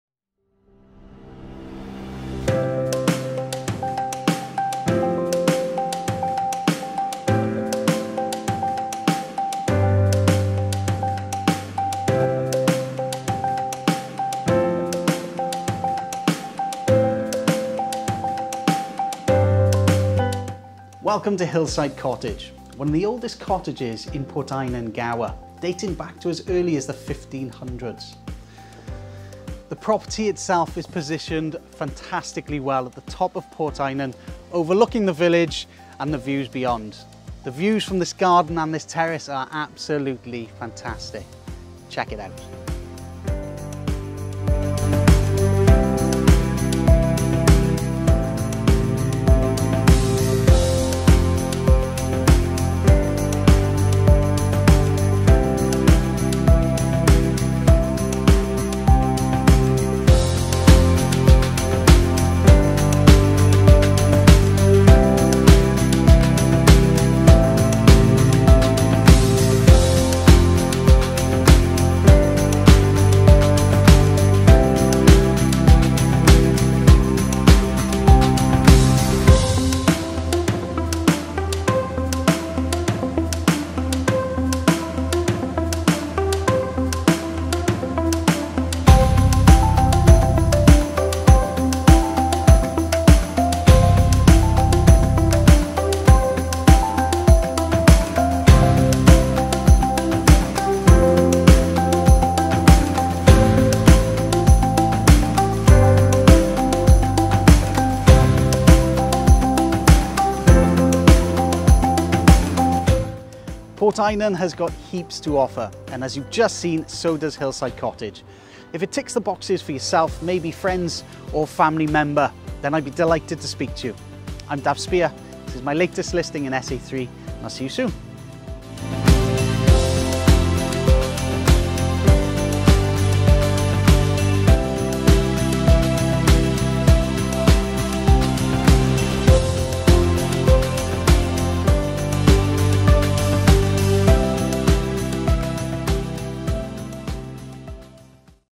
Hillside Cottage, Port Eynon
A charming FOUR BEDROOM COASTAL COTTAGE which has been extended and offers a serene & picturesque retreat for those seeking a TRANQUIL LIFESTYLE.  Nestled into the elevated countryside, with a tiered floorplan & garden, the home features PANORAMIC SEA VIEWS and a scenic position above the village. Bishopston School catchment area.

Featuring THREE VERSATILE LIVING SPACES, TWO BATHROOMS, UTILITY & WC.  With origins dating back to the 15th century, comprising vintage features such as stone walls & overhead beams and a cosy logburner.  These gorgeous original details contrast brilliantly with the top floor architect designed contemporary extension.  The property is overflowing with charm and character and with sea views from every floor, the home offers a seamless blend of interior and exterior aesthetics.  There's ample space in the garden to enjoy the outdoors, either relaxing, dining, entertaining or simply enjoying the sea views in one of the many private areas.  The garage is located to the rear and externally there is a wealth of extra features, including a large storage room.

Located in the quiet, unspoilt village of Port Eynon, Gower, well known for its beautiful beach & coastal scenery. A popular destination for tourists & locals, offering a range of activities, including swimming, hiking and water sports, making it a great place to explore and a fantastic place to call home.  If you're interested in a home within close walking distance to the beach, don't delay, call to arrange your viewing today.